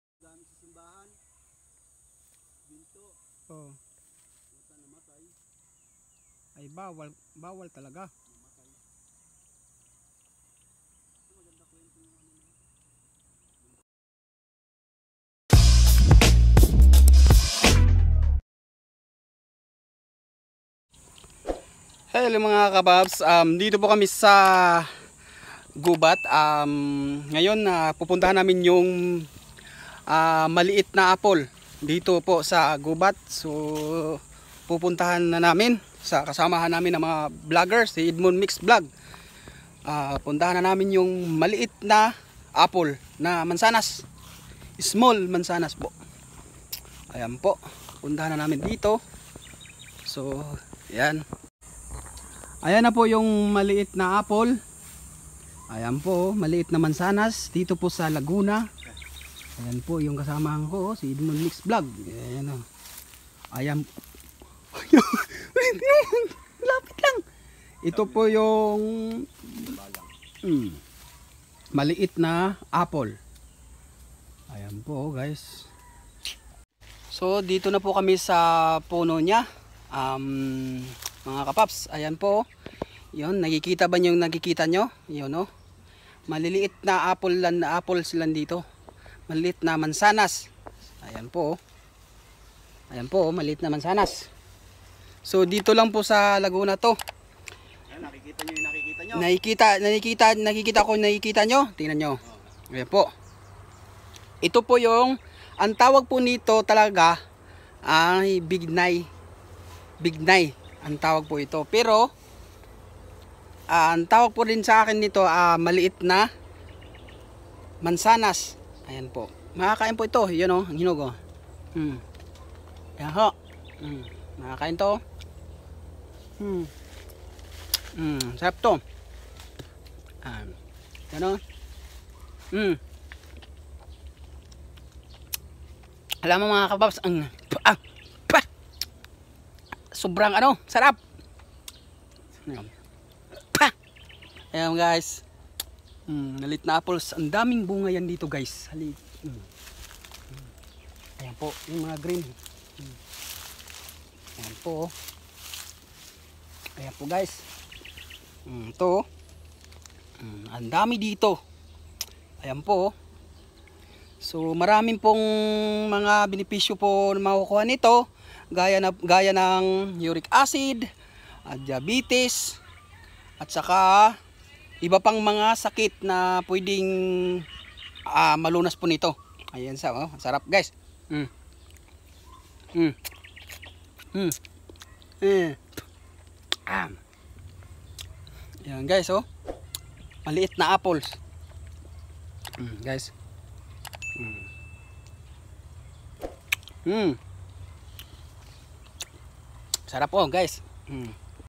Top-one Maliit na apples in Laguna | part 2 Tara Puntahan natin sa farm.
Description: top one small apples in Laguna so let's go to adventure to the Farm.

Thank you for support my YouTube channel